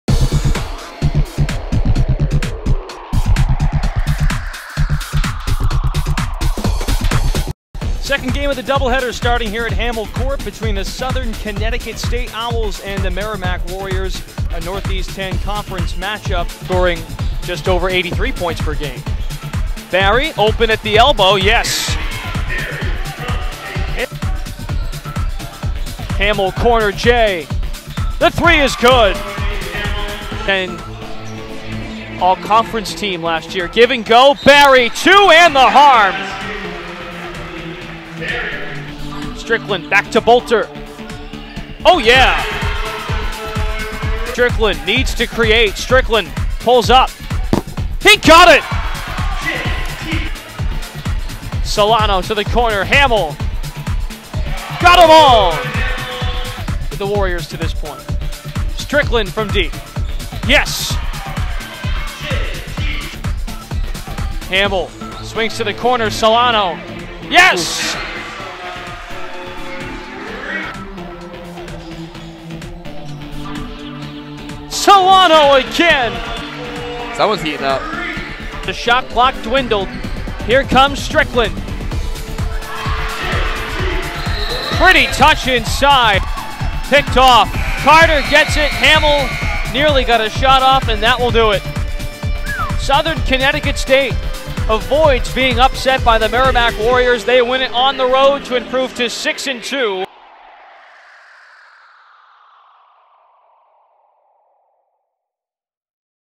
MBB: SCSU Highlights (1-12-16)
Highlights from Merrimack's Education Day showdown with Southern Connecticut.